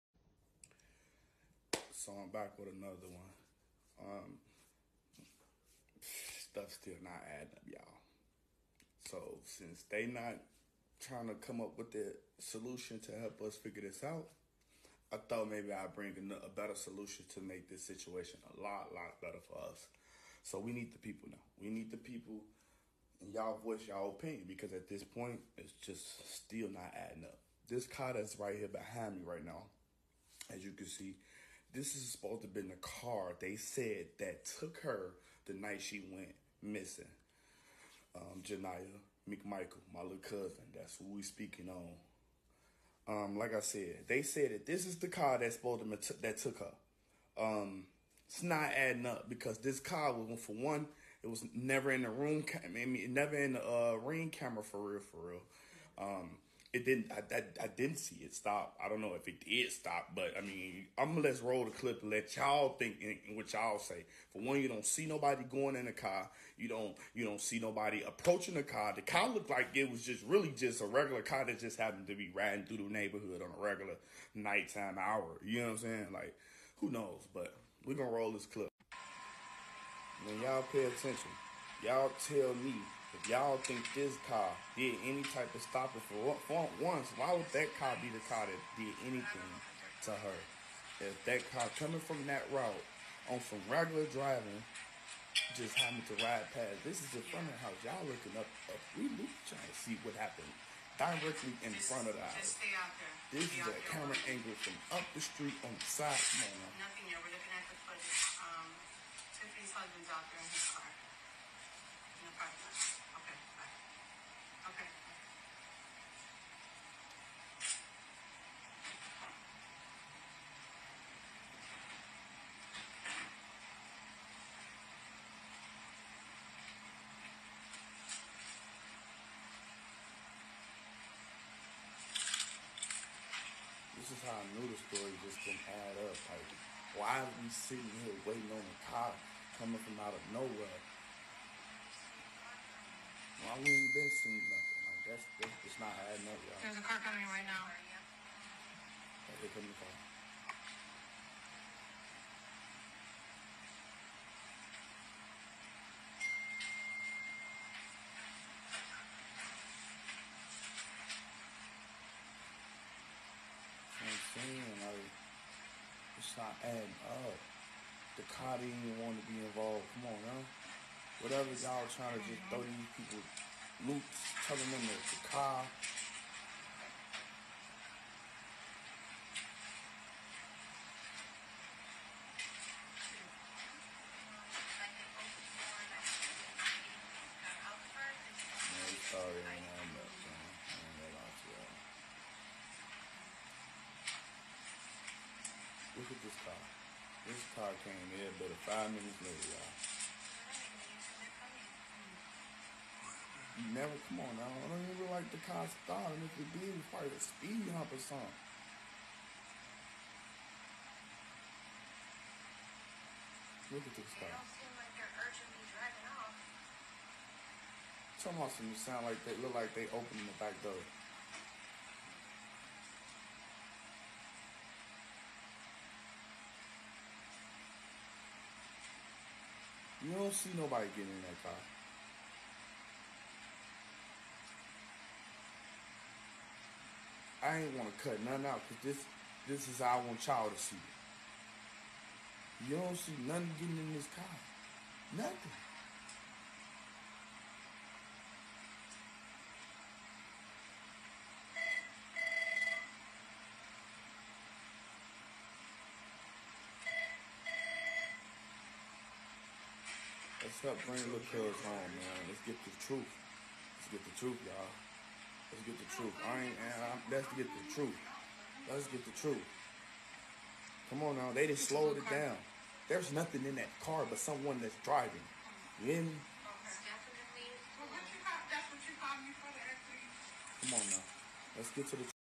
Cousin of missing Gary teen discuss car captured on surveillance footage
Cousin of missing Gary teen discuss footage of car seen driving near her home

By: Missing Lost And Trafficked
      Lured Lost And Unsolved

GARY,Ind.- Ja'Niyah McMichael-Rogers,13 was last
 seen by family members outside
of her home in June of 2024.

Jasmine McMichael,  Ja'Niyah's mother reported the Gary teen missing to the Gary Police Department
on  on an unspecified date the second week of August.

McMichael allegedly told multiple family members and Kevin Collinz CEO of KC Body Guard Services,that she noticed her daughter was not at home around 12:47 a.m. when she suddenly woke up to find her front door ajar on August 11,2024.

Jasmine told ABC 7 Chicago
when she got up early the next morning to get the kids up for school, she found the home's front door ajar, the back door unlocked, and her oldest daughter gone.

It's uncertain if McMichael is confused regarding the day she last saw her daughter, or intentionally giving false statements.

The first day of school for students in Gary, Indiana in 2024 was Wednesday, August 14, from 7:45 AM–3:30 PM.

Ja'Niyah's cousin released surveillance footage showing a car driving at a slow speed near Ja'Niyah's home around the time of her disappearance.
The car doesn't appear to make any sudden or gradual stop.

Missing Lost And Trafficked was told by a family member of Ja'Niyah that the vehicle captured on surveillance footage could clearly be seen stopping, with a passenger , and
after stopping a car door was
opened.

The home where the teen was last seen is located 1900 block of Malcom X Drive, Gary police said. She is  about 110 pounds, and she was last seen wearing black pajamas, a black hooded sweatshirt and black and
red shoes allegedly.

Submit tips and story ideas to